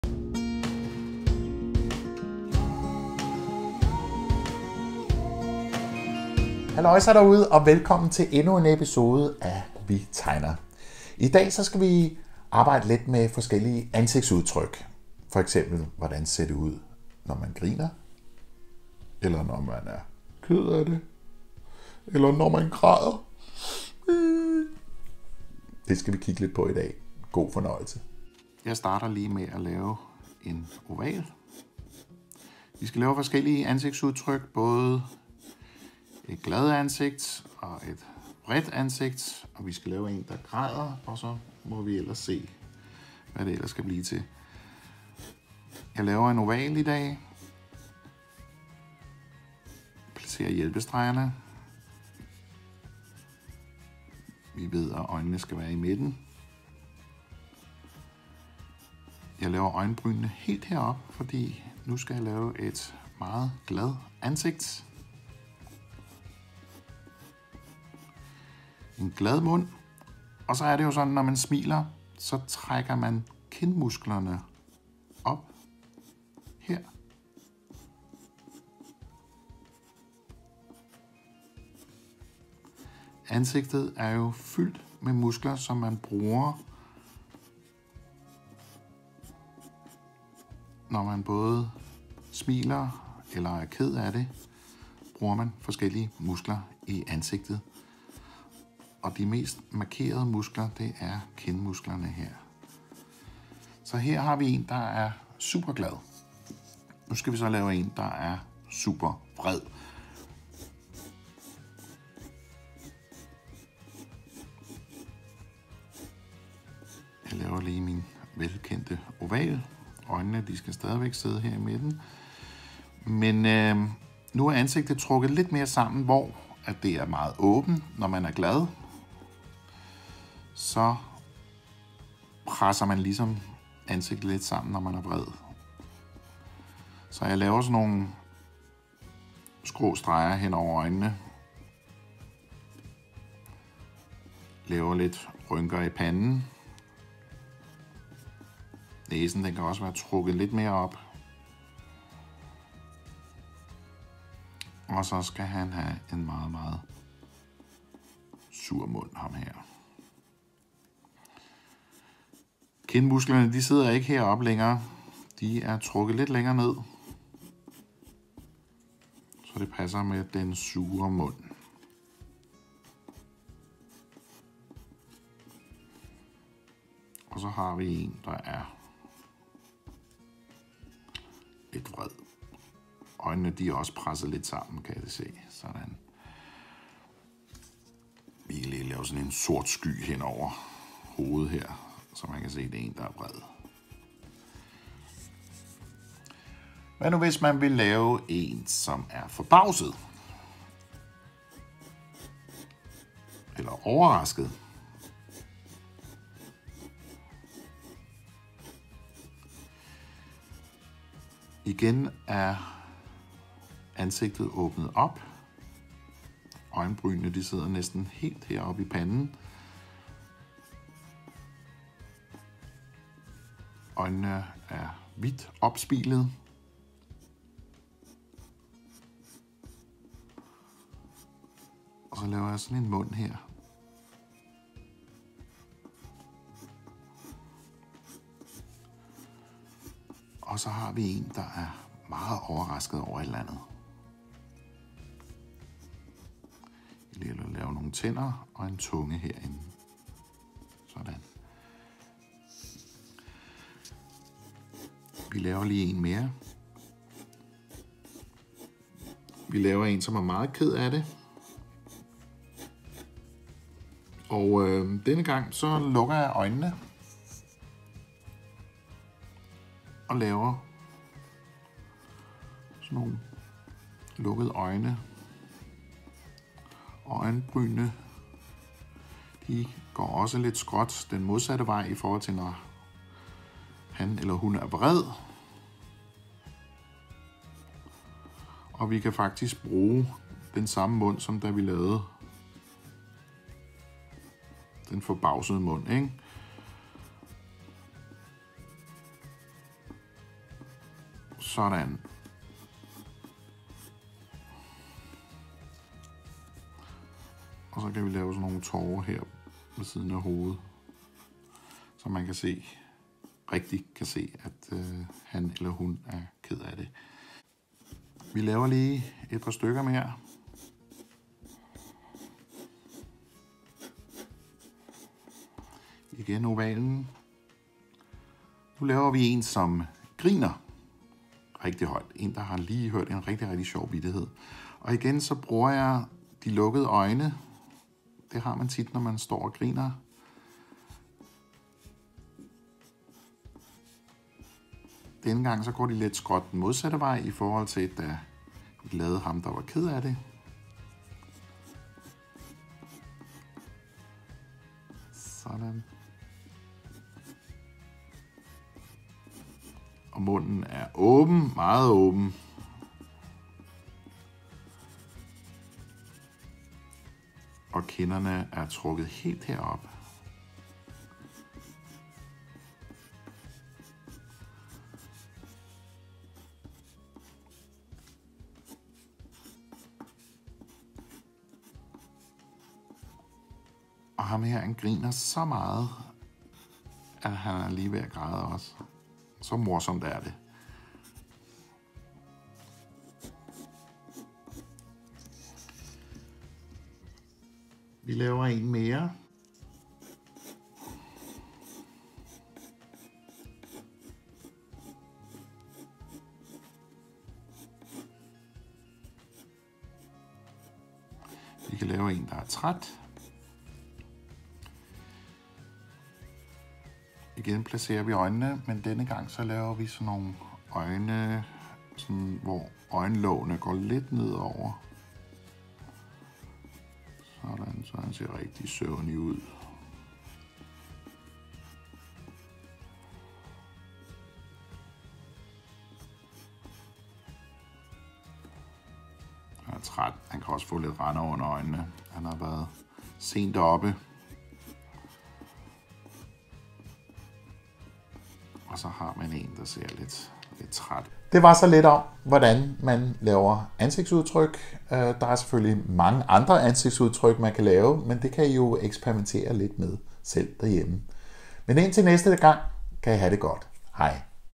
Episode 9: Ansigtsudtryk
Glad, sur, ked af det, grinende, træt. Se med hvordan man nemt og hurtigt kan tegne de forskellige udtryk.